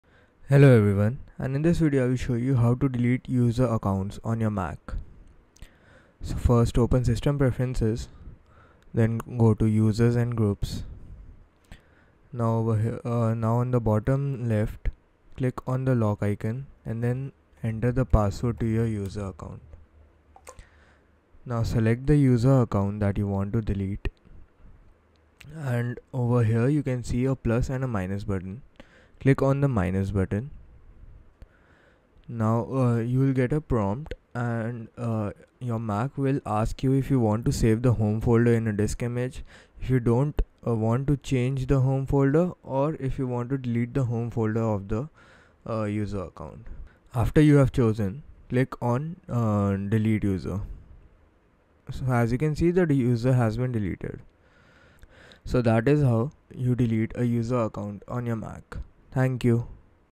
How To Delete User Accounts On Mac
In this video we will see How to  Delete user account on your Mac.  So let use see How to Delete a User Accounts to Your MacBook .